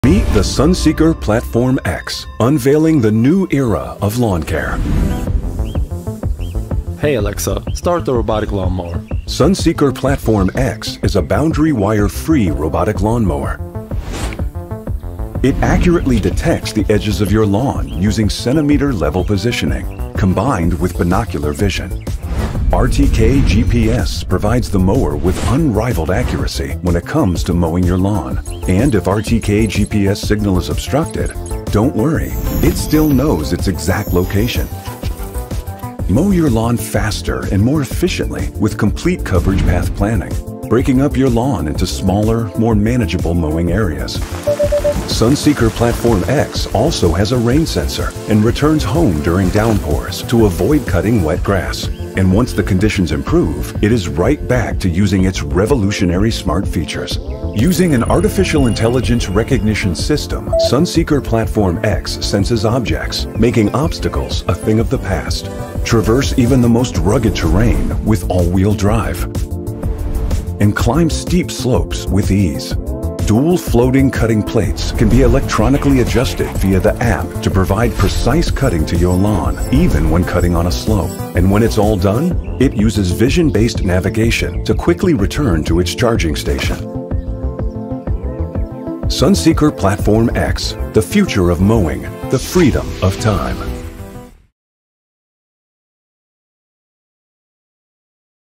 Introducing The Sunseeker Orion X7 Wireless AWD AI Robot Lawnmower
@MacBrothersLimited would like to introduce the Global Award Winning Orion X7, by Sunseeker.
This Robotic Mower is the leader when it comes to robotic mowing, offering an AWD system that can tackle even the toughest of slopes, while also working on a zero turn, preventing damage to your lawn while also getting into tight spaces.
Driven by GPS Satellite and AI Vision Learning, this mower sets itself ahead of the rest in the market.